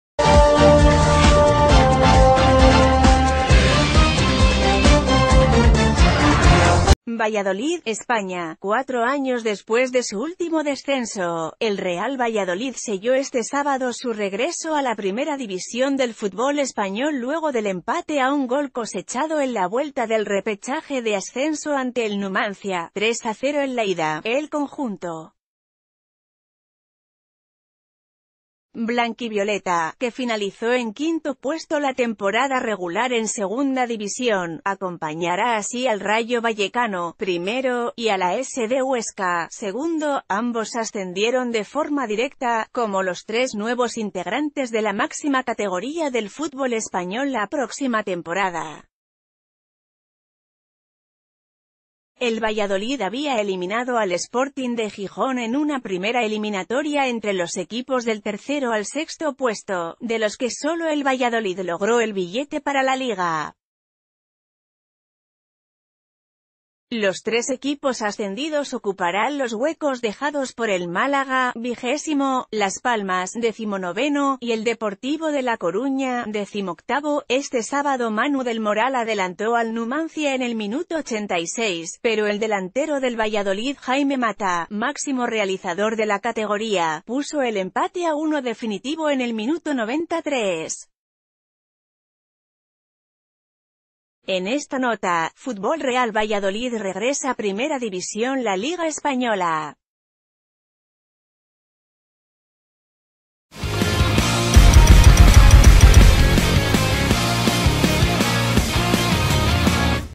El Valladolid asciende a primera división tras superar al Numancia
Valladolid, España.- Cuatro años después de su último descenso, el Real Valladolid selló este sábado su regreso a la primera división del futbol español luego del empate a un gol cosechado en la vuelta del repechaje de ascenso ante el Numancia (3-0 en la ida).El conjunto blanquivioleta, que finalizó...